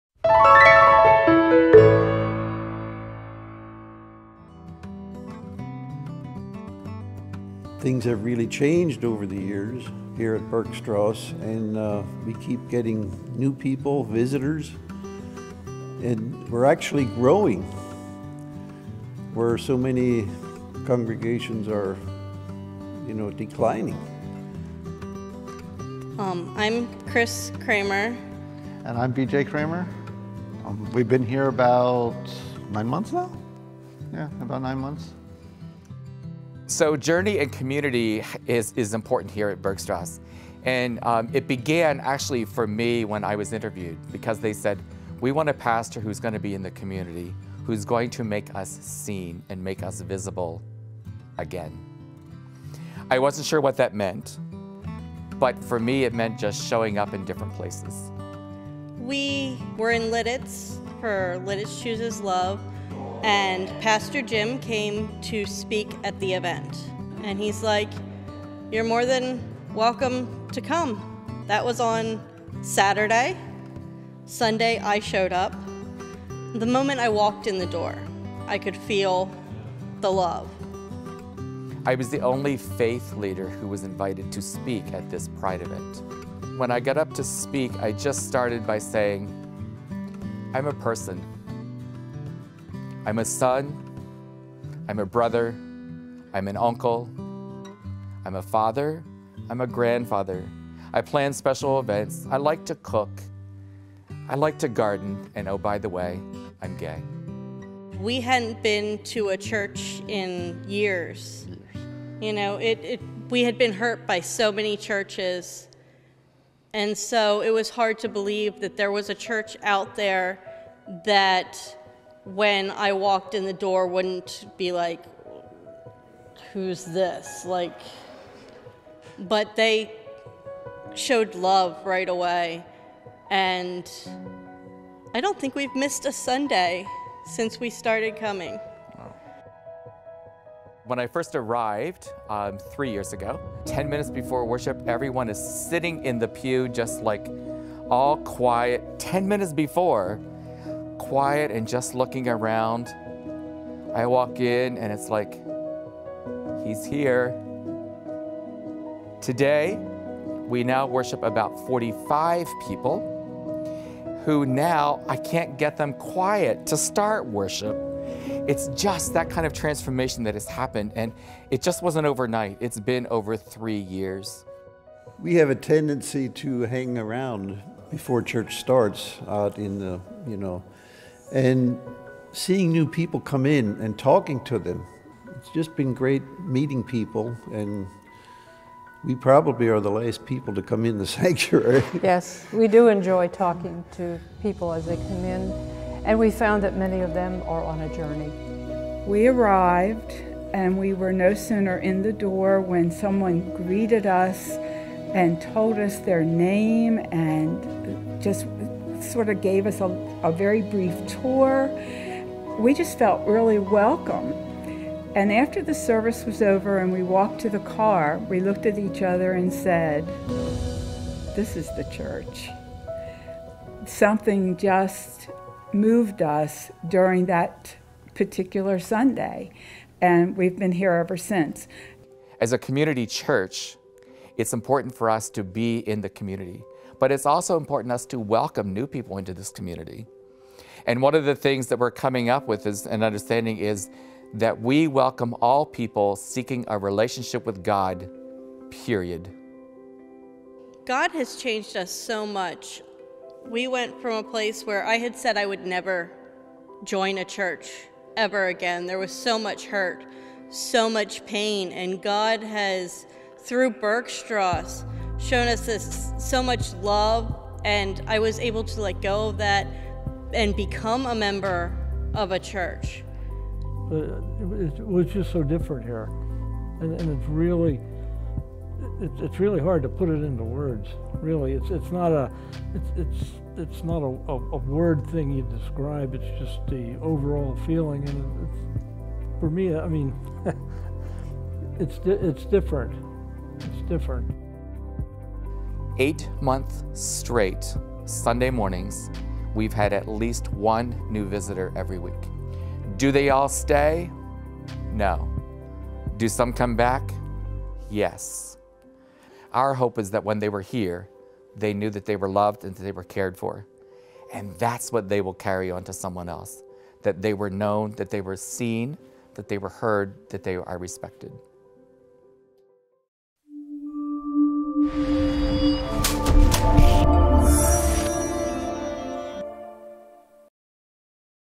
Bergstrasse Lutheran Church is Journeying With Jesus - Synod Assembly 2022 Key Mission Video
Rev. Jim Goodyear and members from his congregation, Bergstrasse Lutheran Church in Ephrata, PA, speak about the journey their congregation has found themselves on in the past few years, taking steps to not only welcome people into their community, but to become a part of the community around them.